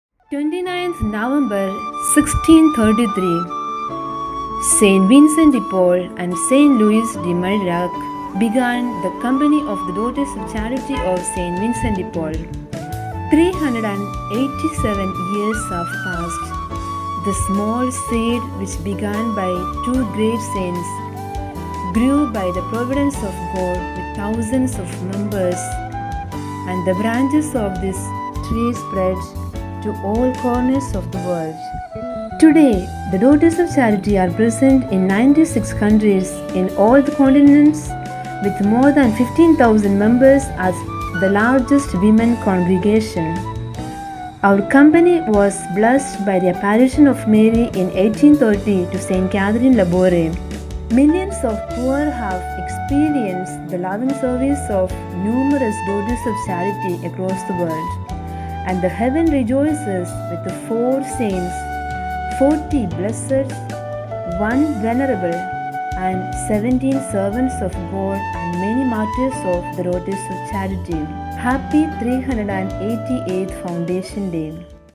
387th foundation day of the Daughter's of Charity of St Vincent de Paul
Prepared and presented by Sr Soniya DC